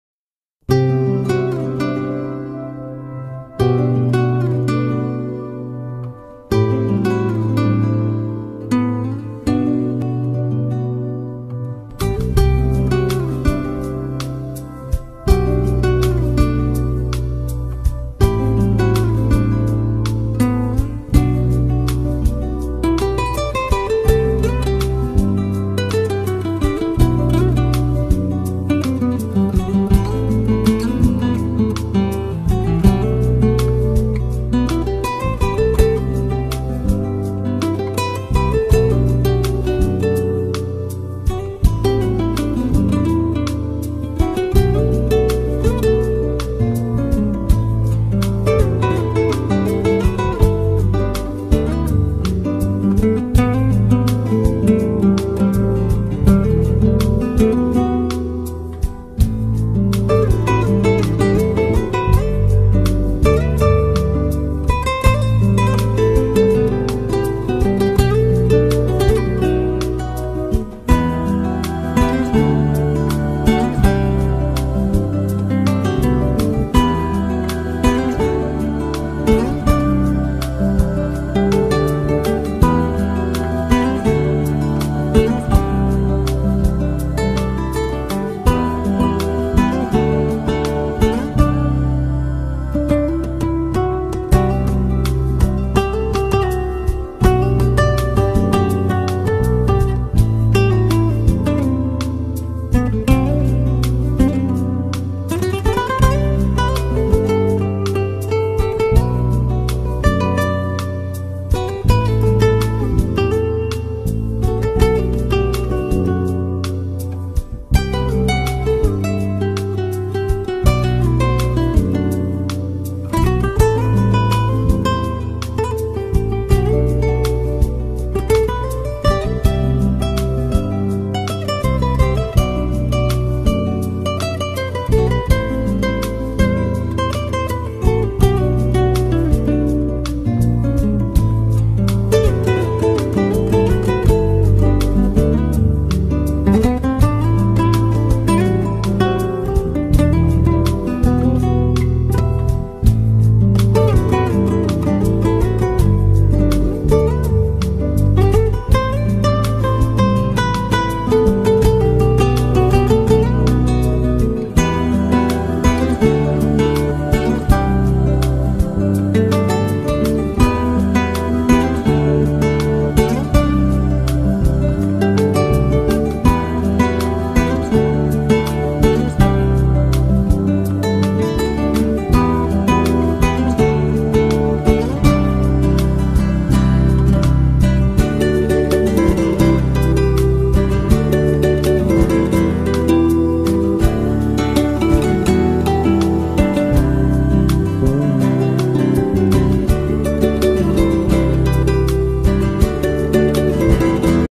The process of drawing a pattern copied on wood with a pencil & dibujar un patrón en madera #2
Now draw a line with a pencil on the copied pattern. you need to draw beautifully without rushing when drawing. then it is carved according to the drawn pattern. If our pattern is beautiful, it will be a work of art that will attract a beautiful person ...!